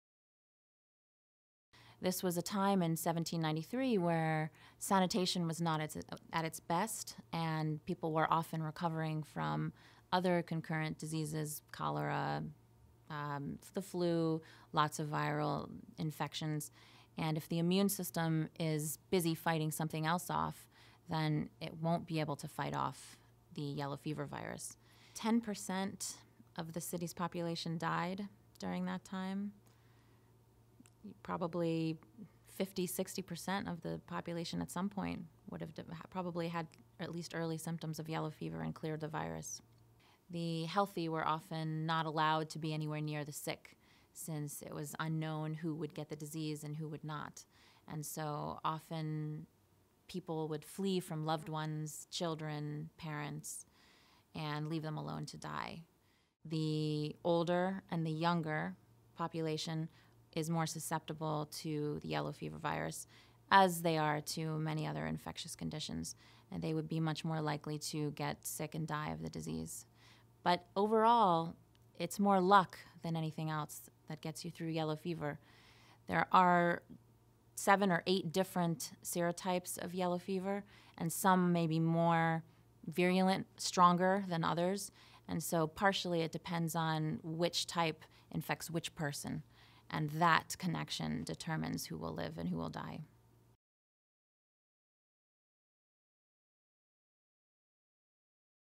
Helen Koenig on Luck of the Draw in Surviving Yellow Fever
Philadelphia: The Great Experiment (Fever)

Helen Koenig explains that the high mortality rates associated with yellow fever made survival a matter of luck during Philadelphia's epidemic of 1793.